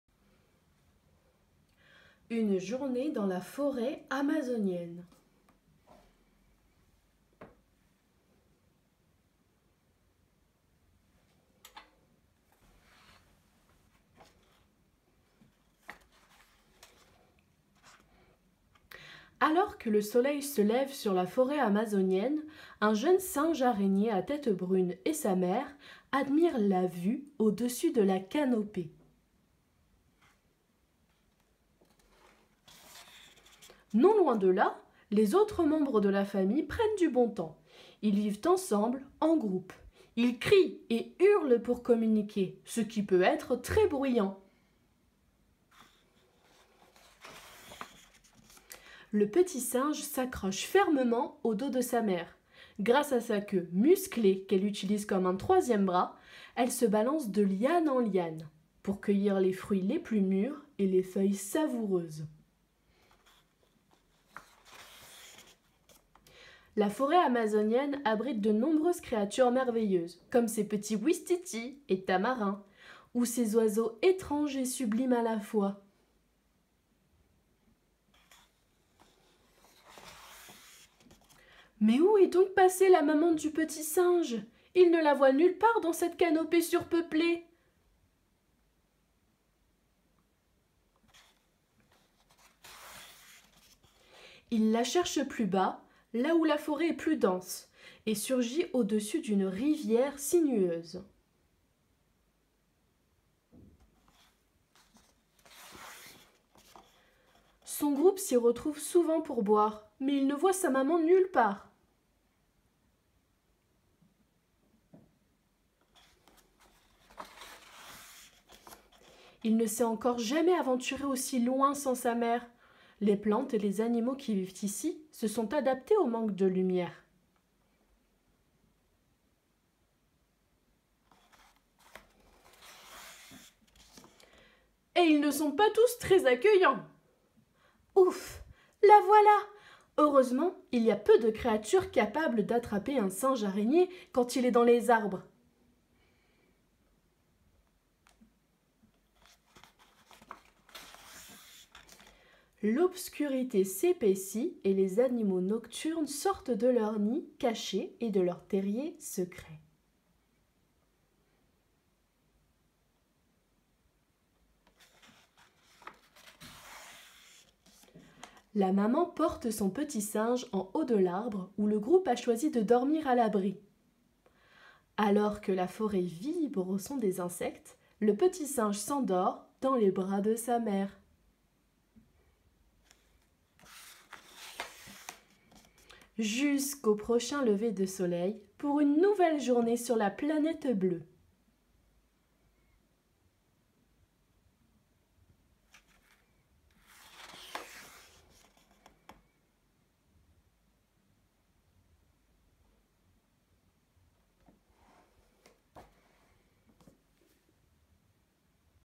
Lecture : Une journée dans la forêt amazonienne
Lecture de l'album "Une journée dans la forêt amazonienne" d'Ella Bailey.

Vidéo destinée à mes élèves de MS/GS.